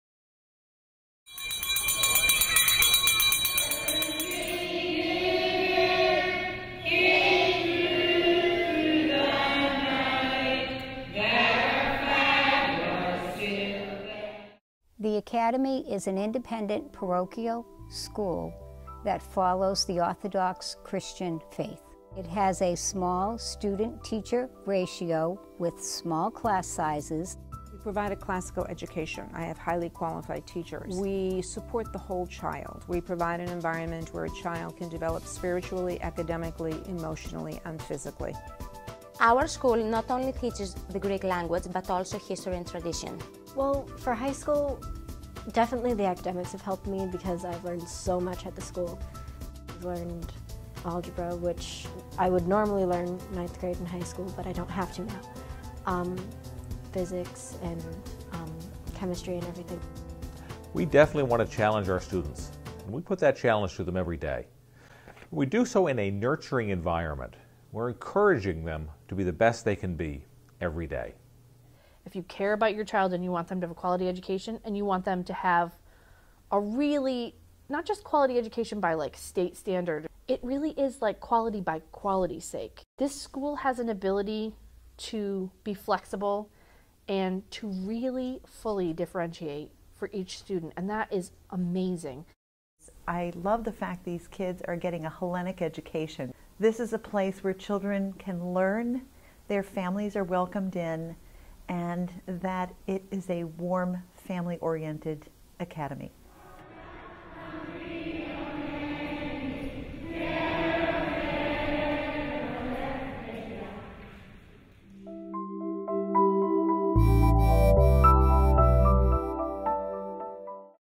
Hellenic American Academy School Video - Short
The Hellenic American Academy, founded in 1906, is the oldest Greek Orthodox school in the United States.

We are dedicated to providing academic excellence based on the Massachusetts curriculum frameworks and the common core, and a nurturing environment where students can develop socially, emotionally and physically.